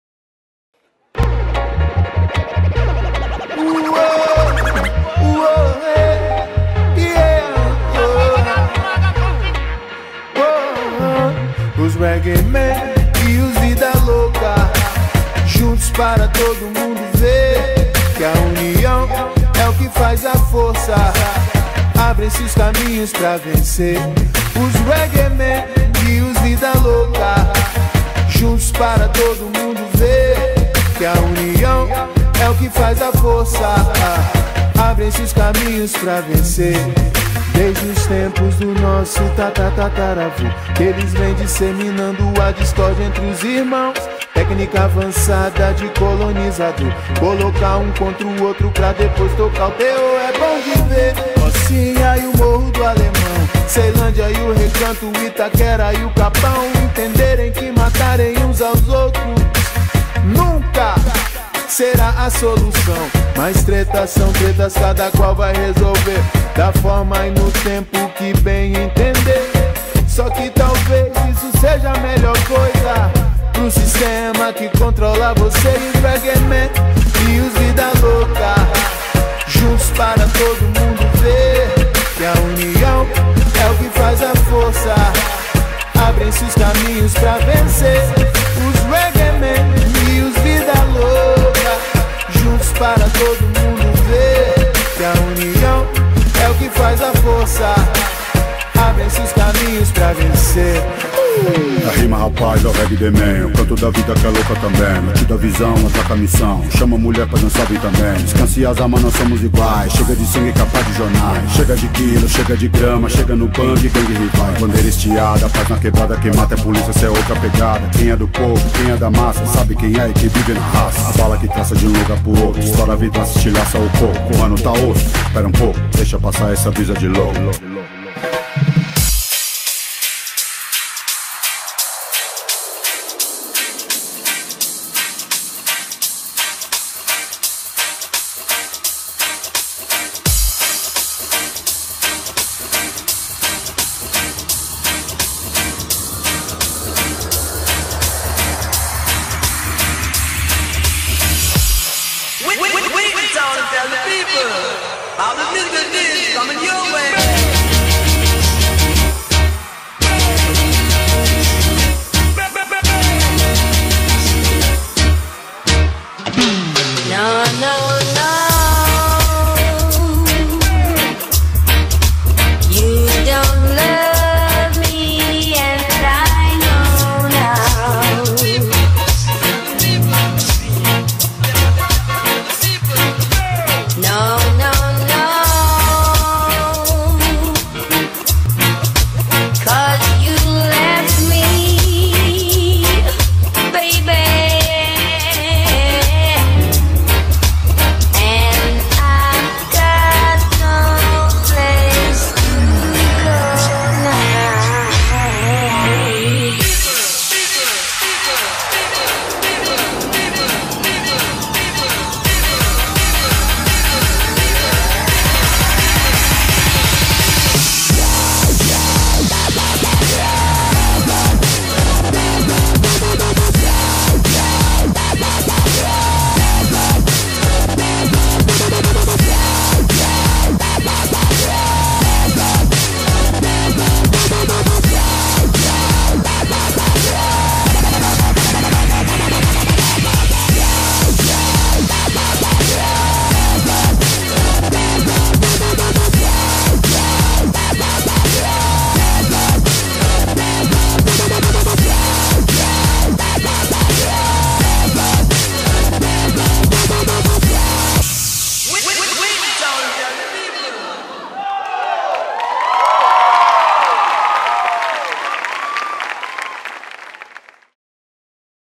"A união faz a força" - Espetáculo "Dito e Feito" - Núcleo de Dança Myller Araújo
Coreografia "A união faz a força" apresentada no III Arte na Rua
Coreógrafo: James Rannon